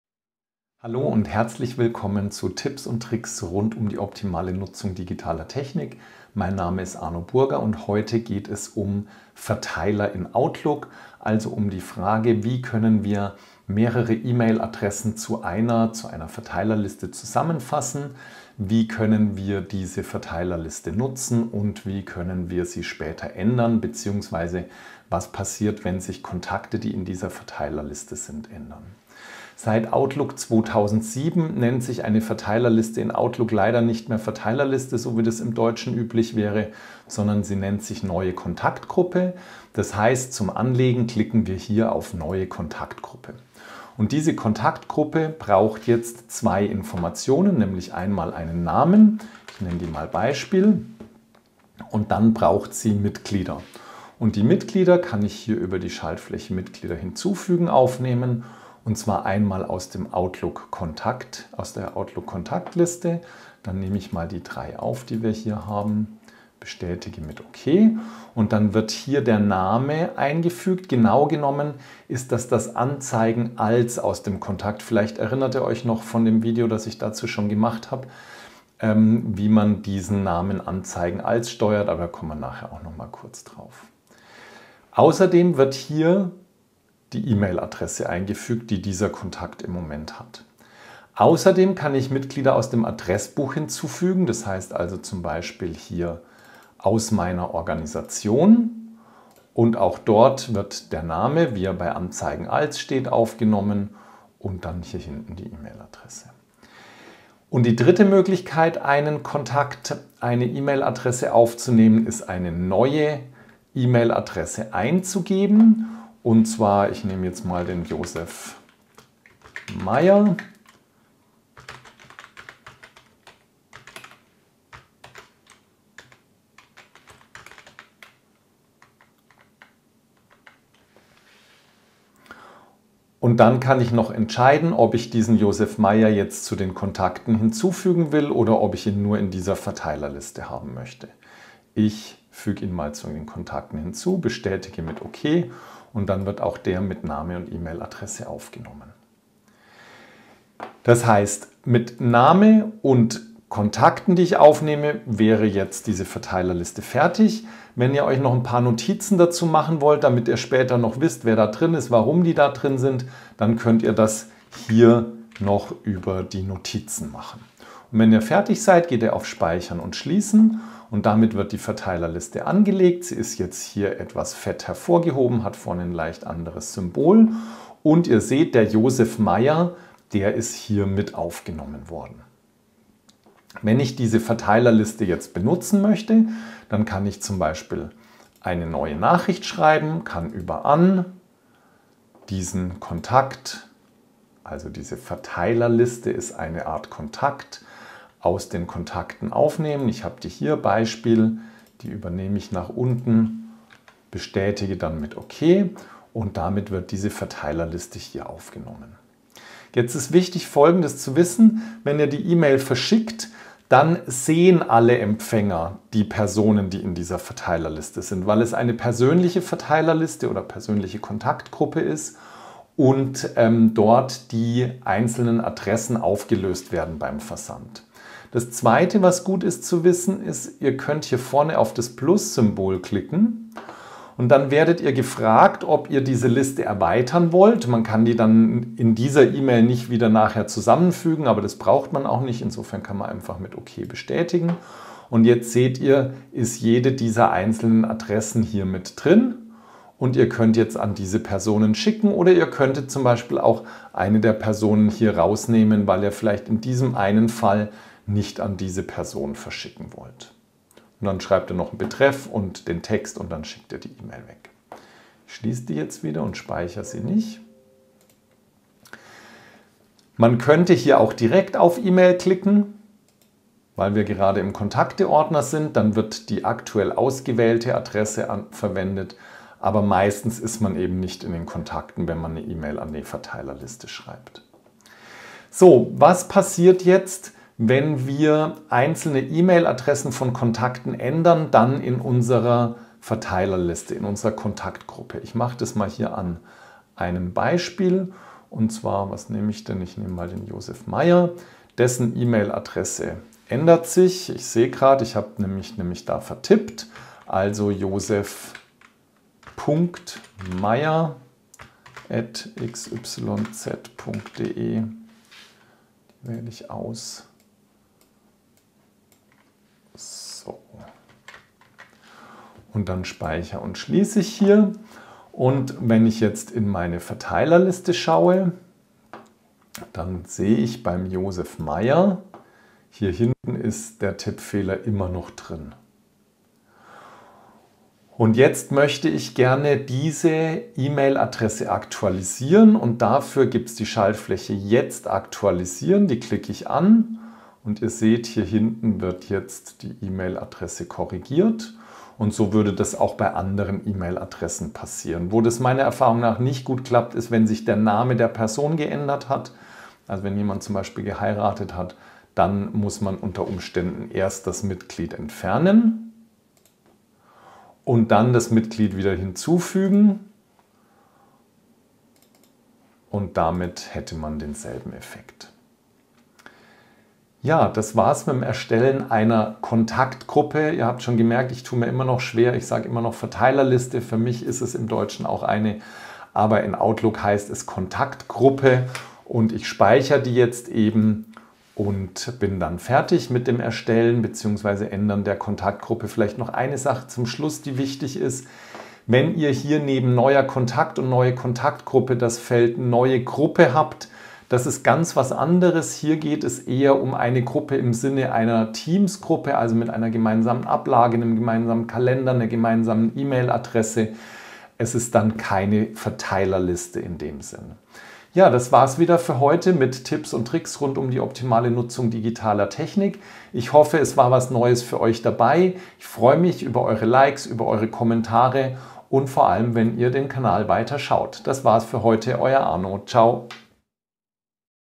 SO erstellst DU eine VERTEILERLISTE (Kontaktgruppe) | Arno Burger | Outlook Tipps und Tricks #6
Verteilerlisten heißen in Outlook Kontaktgruppen. Sie lassen sich sehr leicht anlegen und ebenso einfach nutzen. Dass man sie auch aktualisieren und für den Versand an einen Teil der Personen auch vor dem Versand leicht auflösen kann, wissen viele nicht. Vorsicht: Persönliche Kontaktgruppen sind nicht geeignet, die E-Mail-Adressen der Empfänger zu verstecken. Sie werden beim Versand aufgelöst, sodass man jeden einzelnen Empfänger sehen kann.

Euer Arno

▬ Inhalt ▬▬▬▬▬▬▬▬▬▬▬▬▬▬▬

00:00 Intro und Überblick
00:39 Kontaktgruppe anlegen und E-Mail-Adressen aufnehmen
03:02 Nutzen einer Kontaktgruppe
04:44 Kontaktgruppe ändern bzw. Änderungen in die Kontaktgruppe übernehmen
06:57 Outro